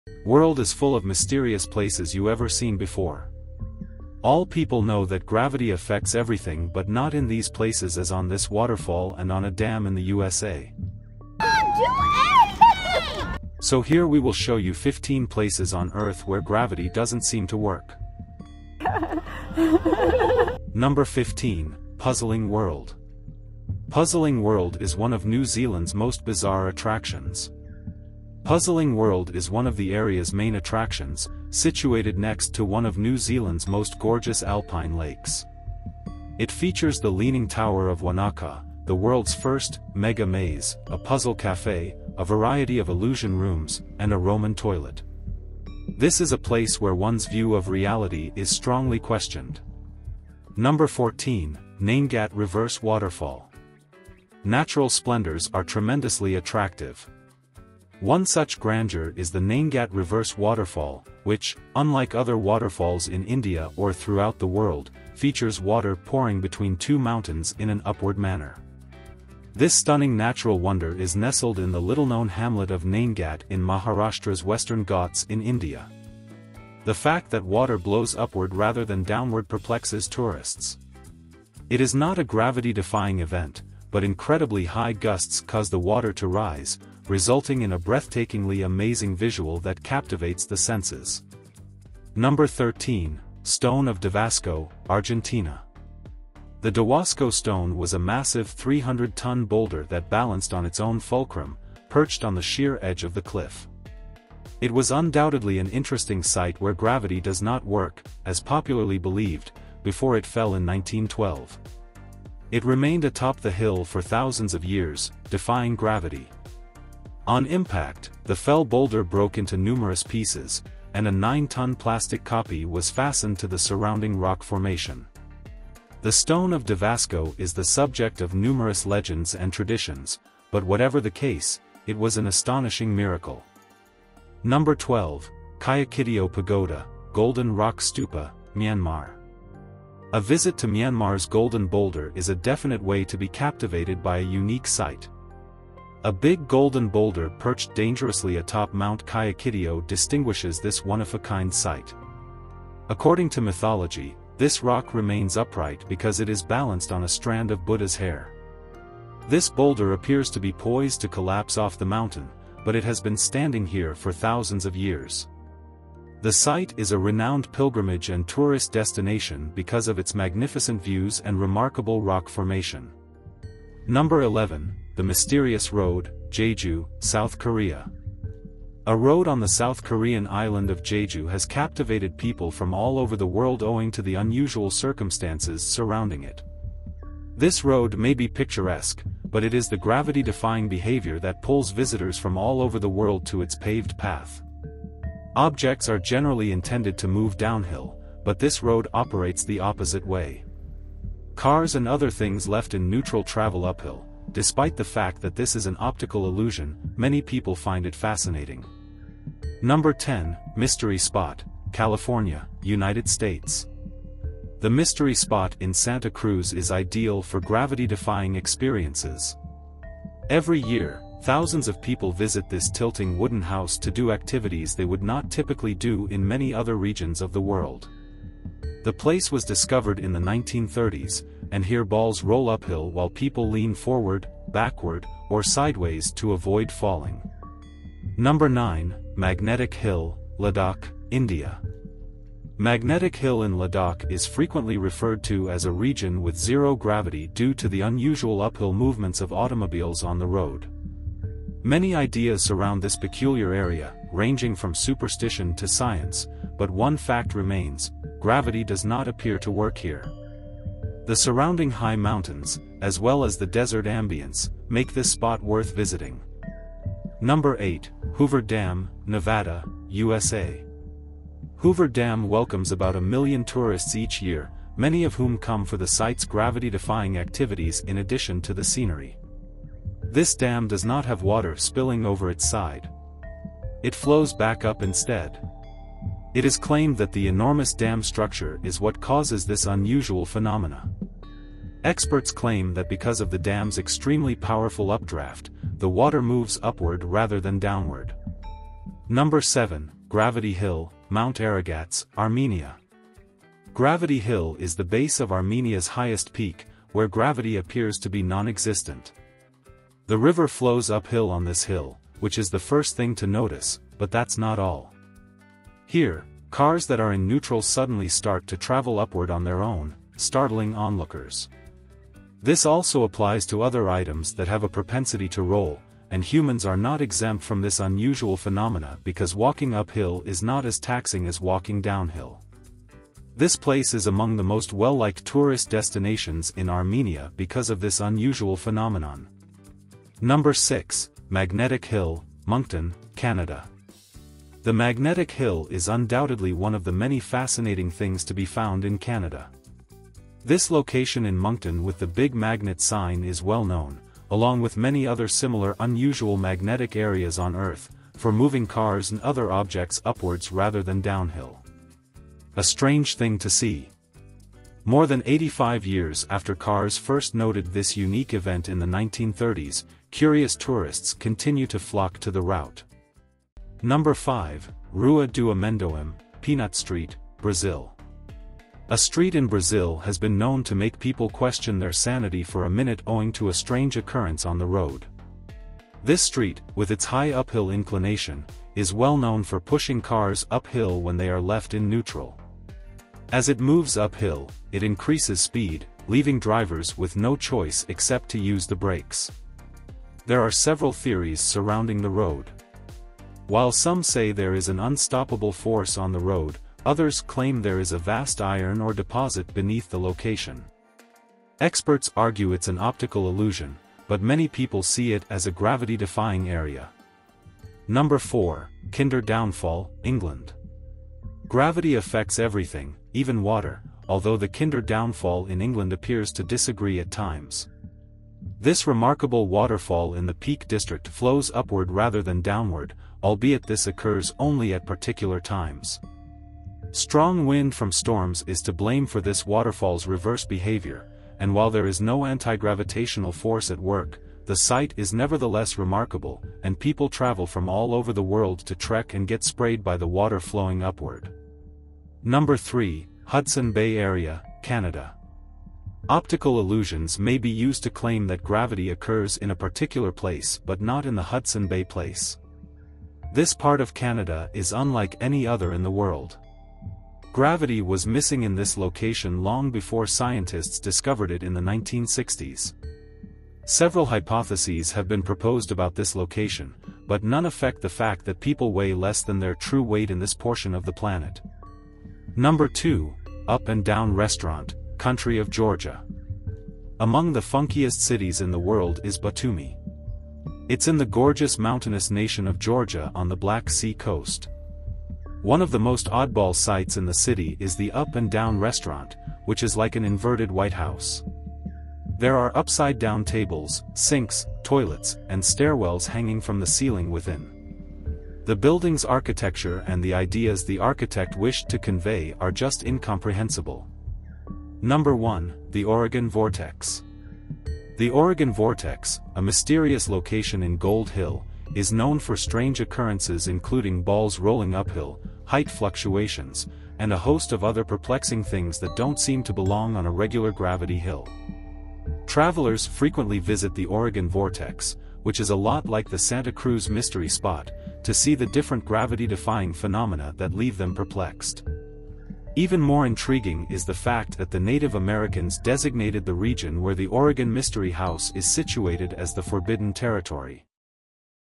15 Places on Earth Where Gravity Doesn't Seem to Work
World is full of mysterious places you ever seen before. All people know that gravity affects everything but not in these places as on this waterfall and on a dam in the USA. So here we will show you 15 Places on Earth Where Gravity Doesn't Seem to Work